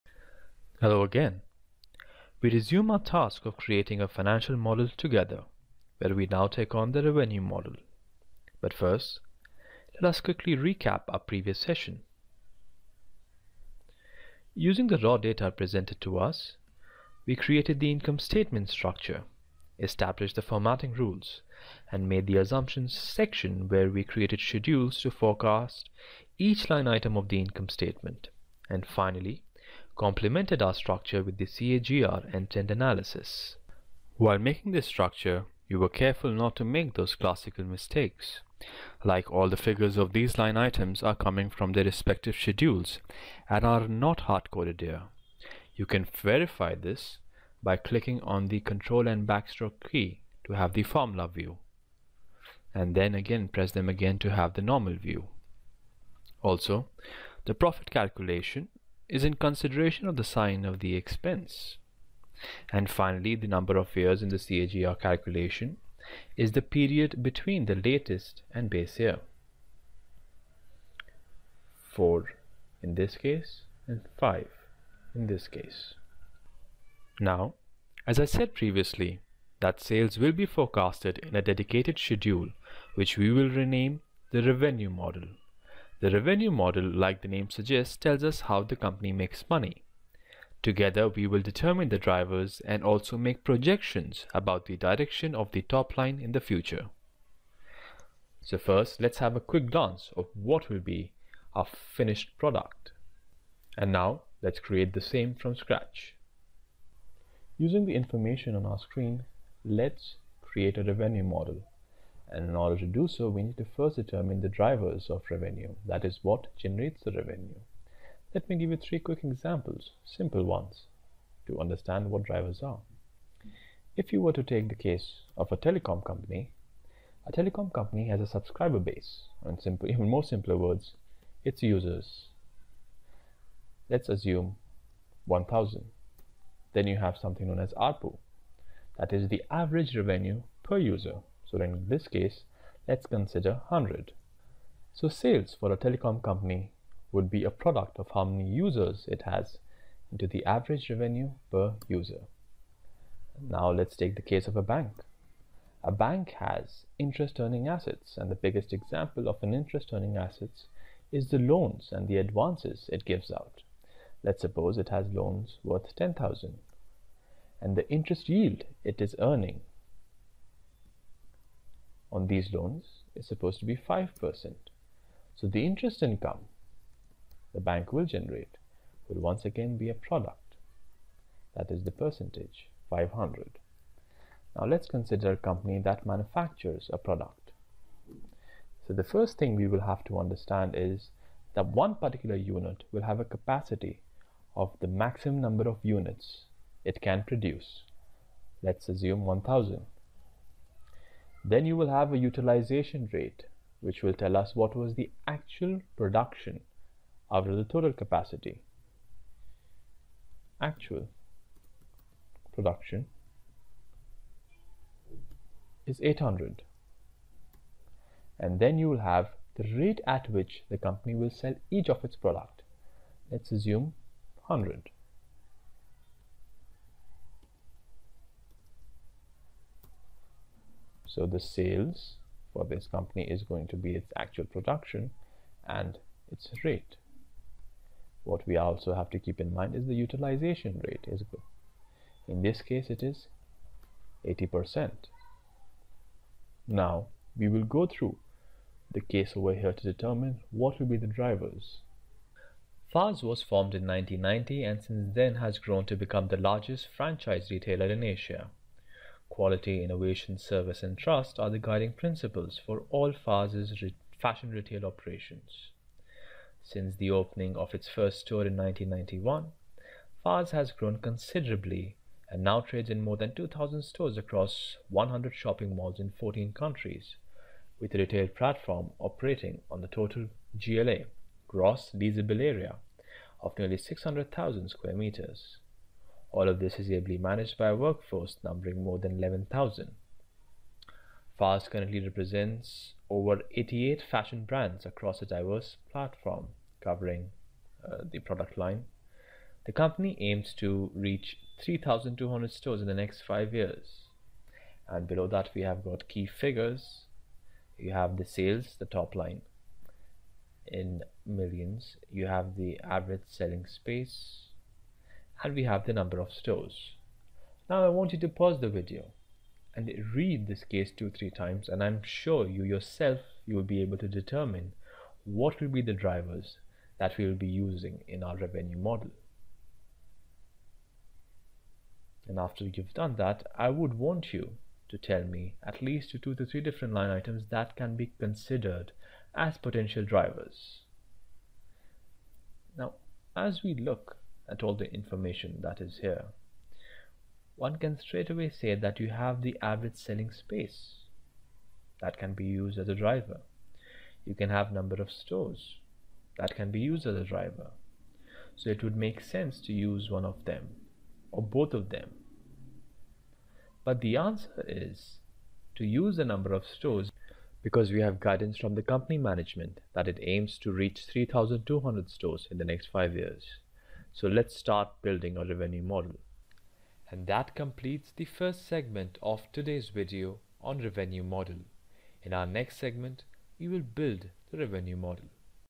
How to Build Revenue Model in Excel with Example | Project Finance 2021
In this video, we will see the Overview of how to make a Revenue Model in Excel with example
we created an Income Statement, Assumption Schedules & CARG that will help us in building the Revenue Model in excel.

With the Help Of the 3 examples, we will show you how the Revenue Model is made -
00:00 Introduction
02:05 Example 1. Telecom Company
02:35 Example 2. Bank
03:05 Example 3. Company Manufacturing any Product

Read the exercise Carefully two-three times and properly understand it
Under the Exercise, they have given some figures
Retail Sales, Total Average Selling Space & Number of Stores Understand the information Properly and Compare it with the Three Examples given Above
This completes the Overview of Revenue Model